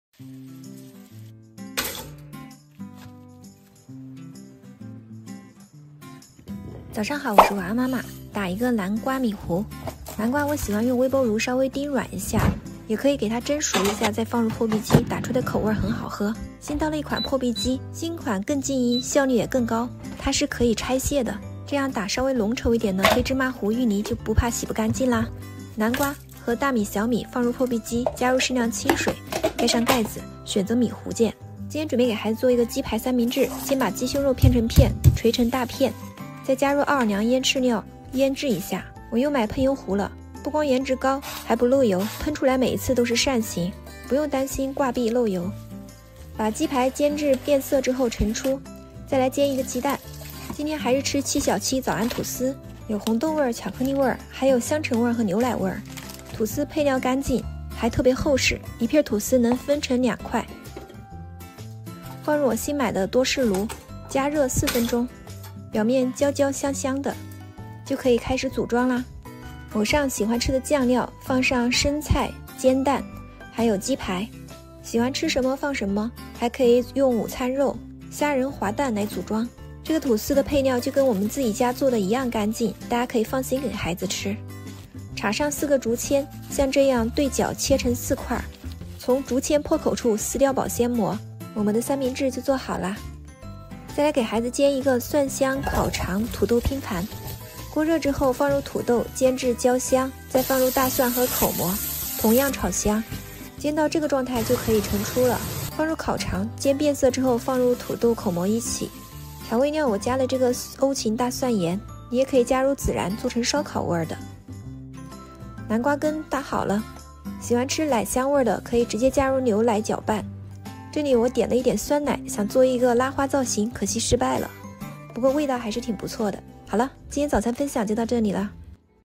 我家早餐从不在外面买，说实话主要是怕我买的厨房神器闲置了
早餐,厨房,Vlog